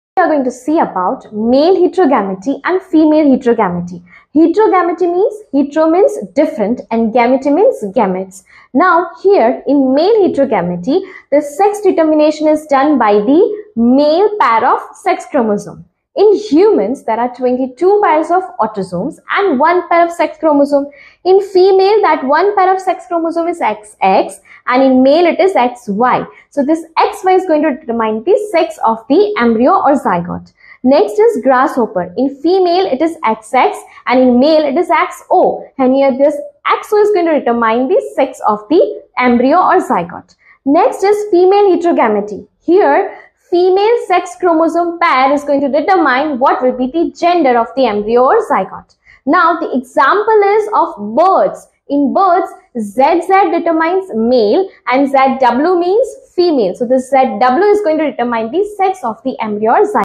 Unveiling Genetic Secrets: Male Heterogamety and Female Heterogamety class 12 Revealed!
In this captivating video, we explore the fascinating concepts of genetics of male heterogamety class 12 and female heterogamety class 12, unraveling the diverse world of sex determination. Join us as we delve into the genetic mechanisms of humans and grasshoppers, showcasing the intriguing differences in their sex chromosome systems.

🔍 Video Highlights:
- Male Heterogamety: Decoding the XY System in Humans
- Female Heterogamety: Unraveling the ZW System in Birds
- Examples: Exploring Sex Determination in Humans and Grasshoppers
- Genetic Complexity: Unveiling the Diversity of Sex Chromosomes
- Discovering the Secrets of Sex Determination in Nature

Join us on this eye-opening journey for class 12 and NEET aspirants into the world of sex determination and uncover the hidden genetic wonders of male heterogamety and female heterogamety. Get ready to be amazed by the intricate mechanisms that shape the diversity of life!